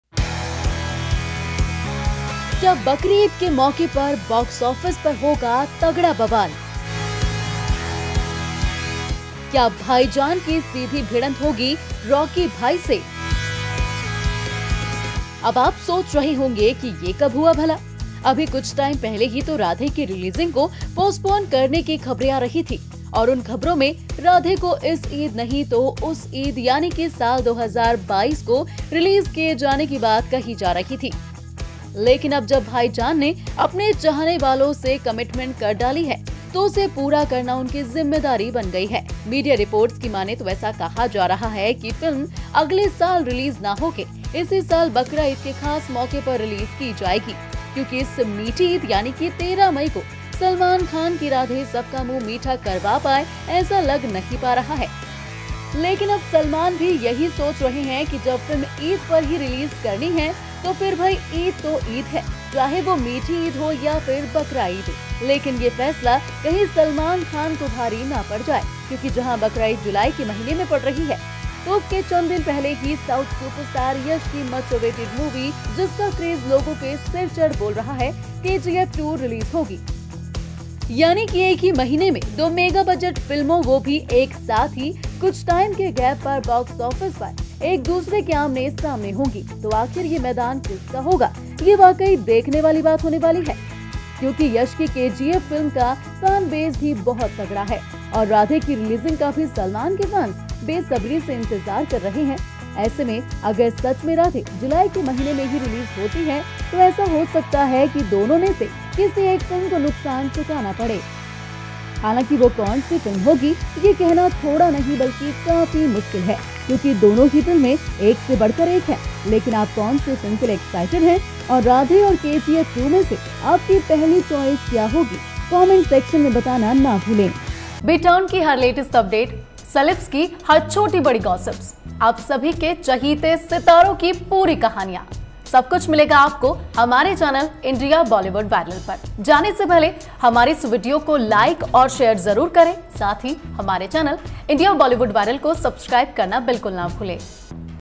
बकरीद के मौके पर बॉक्स ऑफिस पर होगा तगड़ा Clash! राधे या रॉकी कौन मारेगा बाज़ी?.
जुलाई के महीने में गर्माएगा बॉक्स ऑफिस का पारा, राधे या फिर रॉकी? किसका बजेगा ढंका